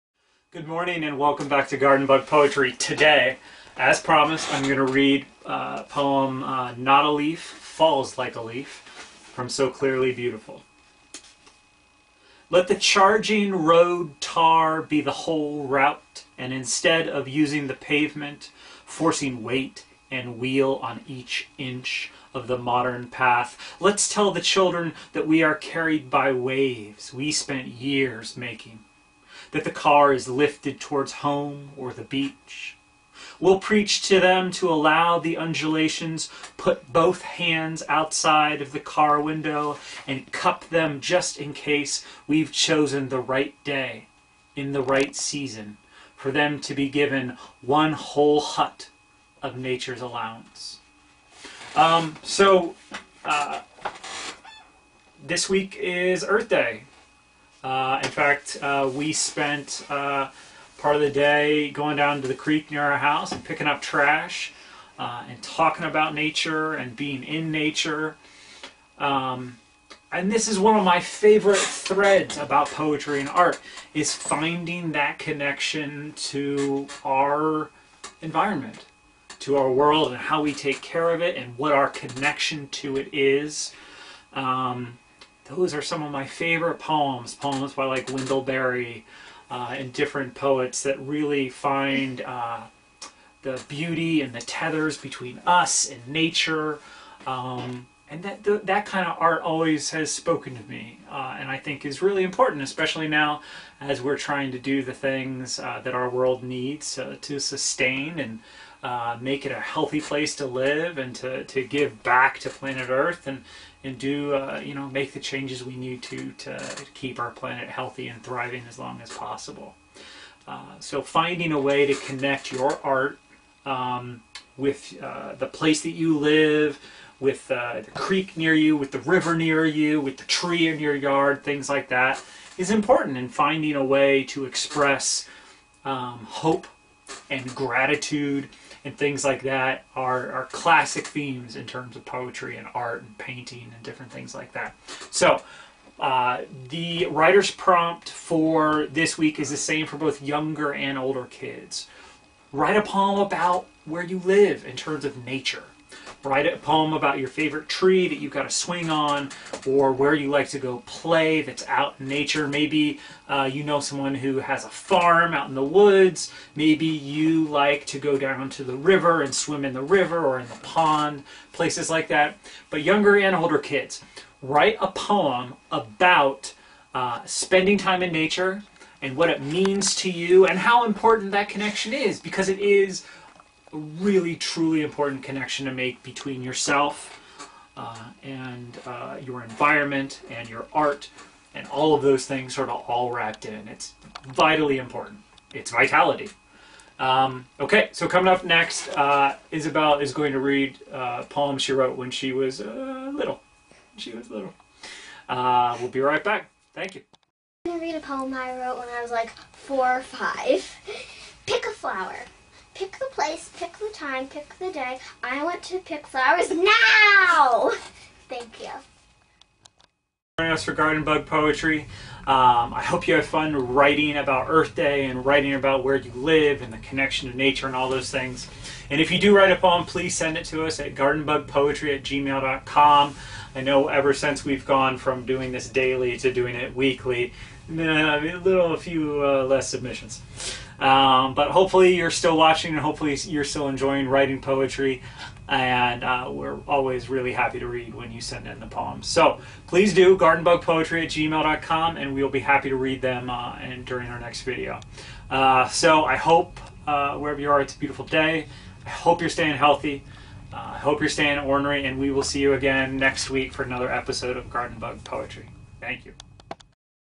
Garden Bug Poetry - Not A Leaf, Falls Like A Leaf
Join host Darren C. Demaree as he reads his poem "Not A Leaf, Falls Like A Leaf", shares a brief lesson about the poem, gives writing prompts for young and older children, and also shares some poetry sent in by kids.